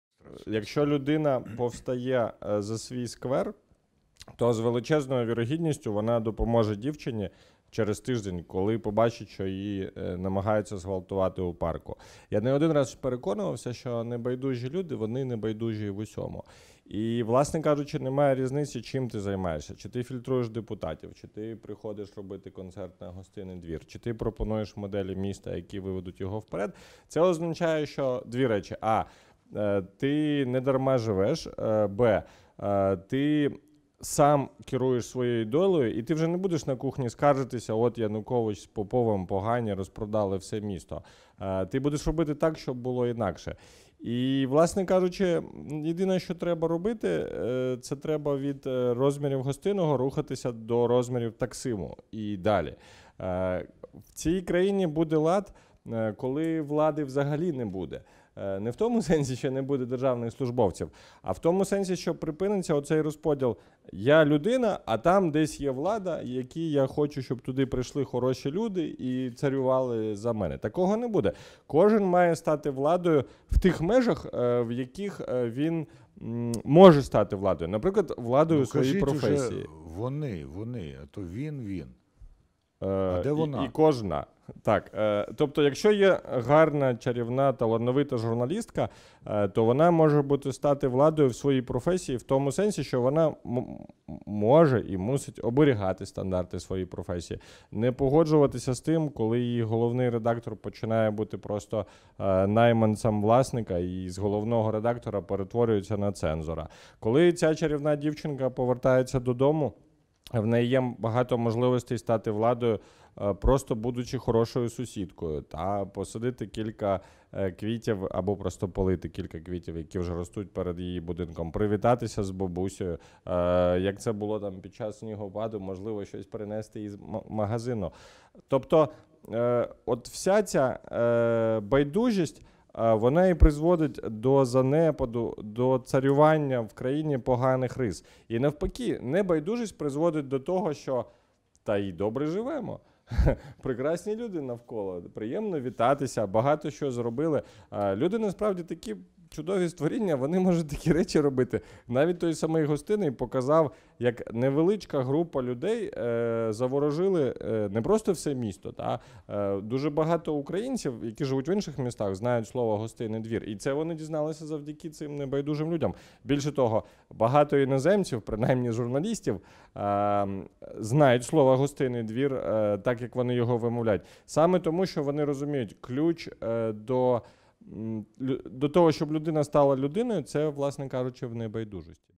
Єгор Соболєв про небайдужість
26 червня 2013 р.
Круглий стіл «Гостиний двір -- Таксим: спільне і відмінне»